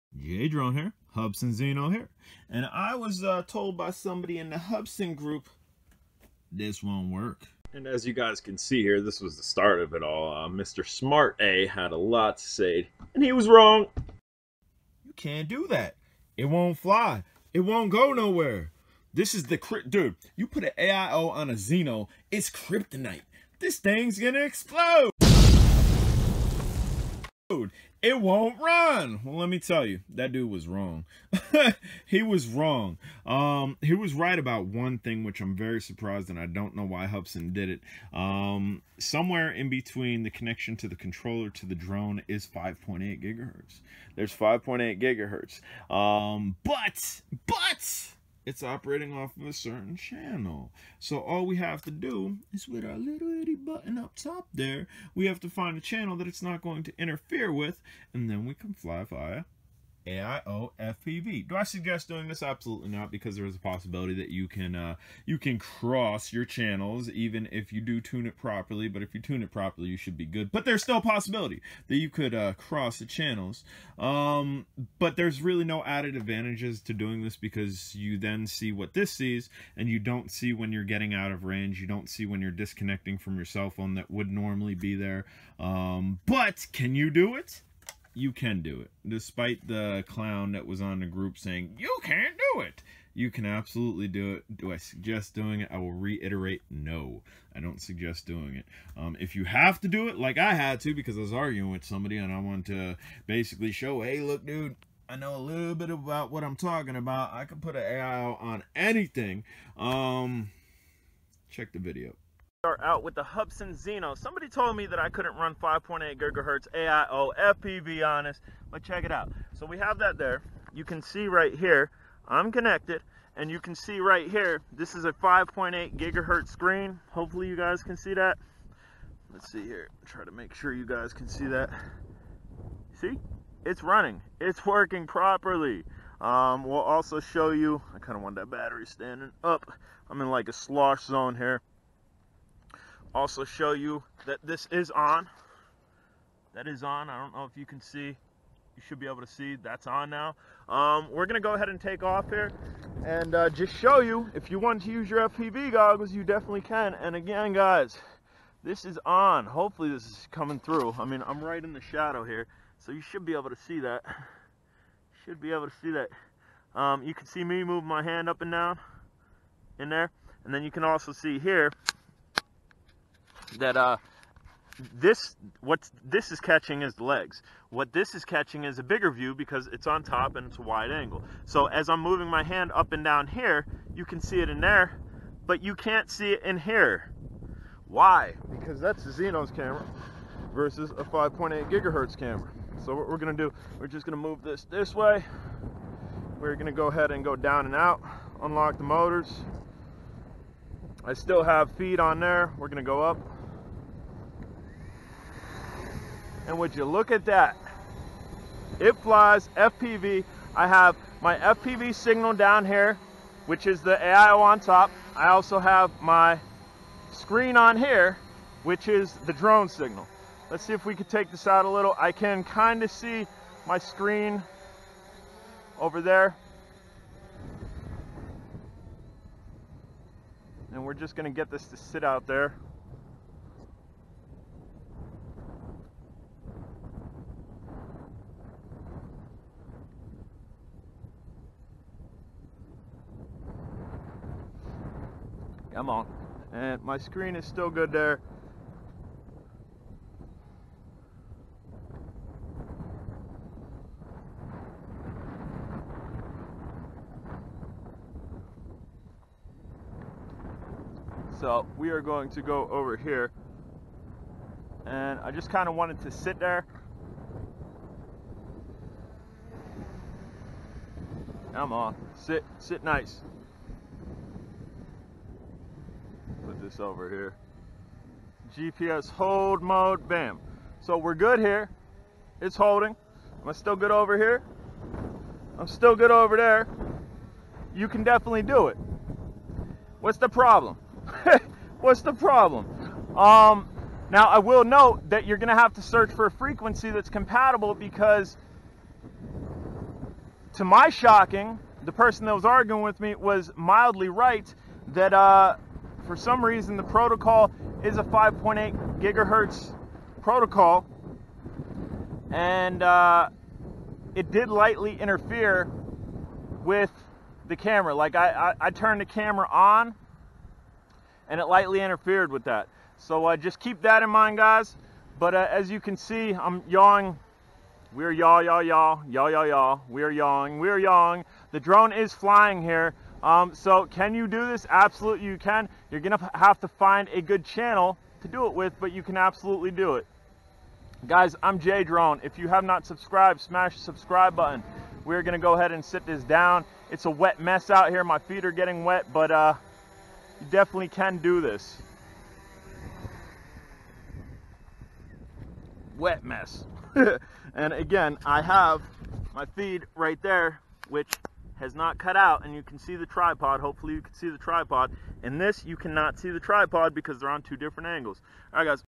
Hubsan Zino REAL FPV 5.8 GHZ GOGGLES Will it work ?
so I was told YOU CANT ADD 5.8 GHZ TO ZINO - Specs